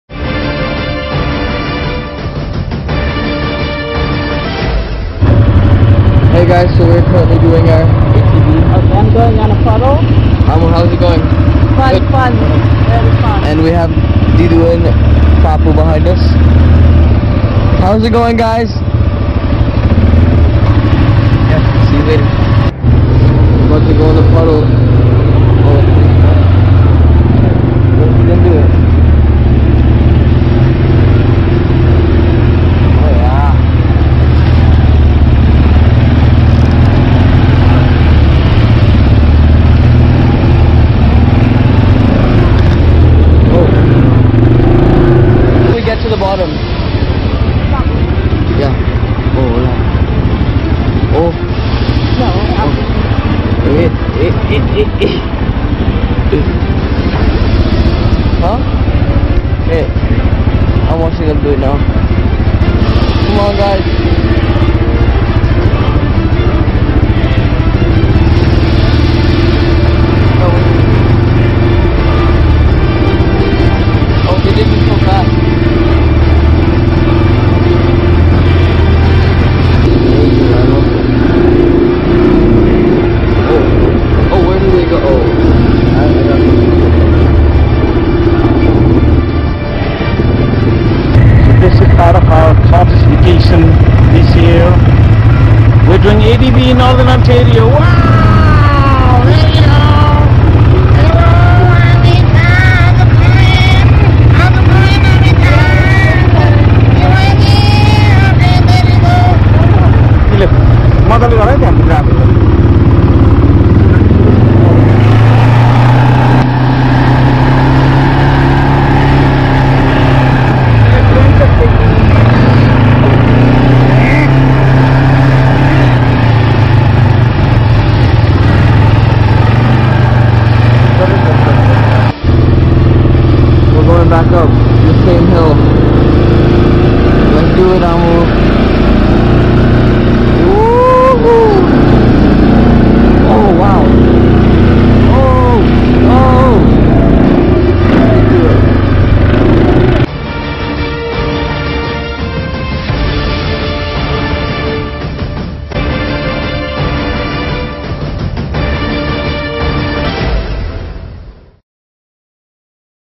ATV ride in Blue Mountains, Ontario 2019
This video is produced by Adon. The event was part of our cottage vacation. Nice.